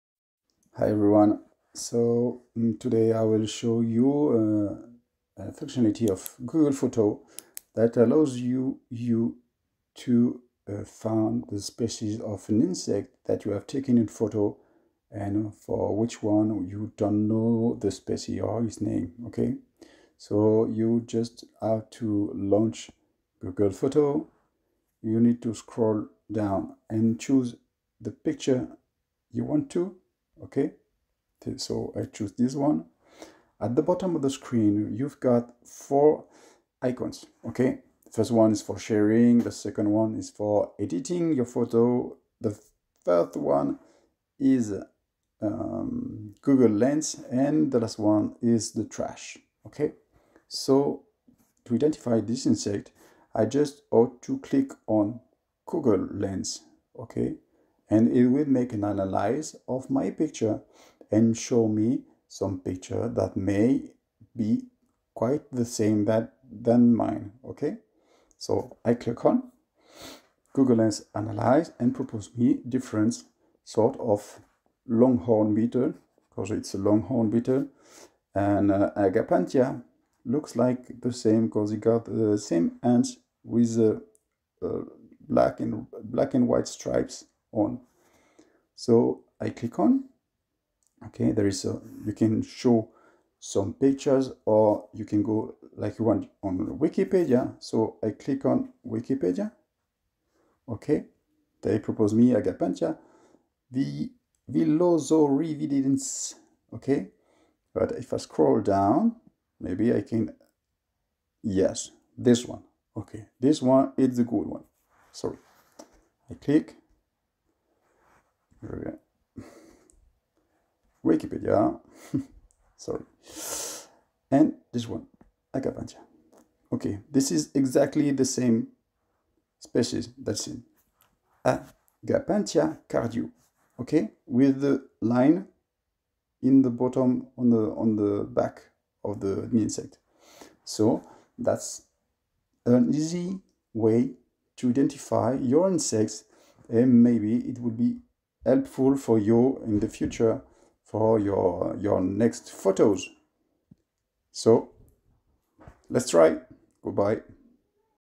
Google lens english
New functionality of google photo that allows you to identify insects on your photos.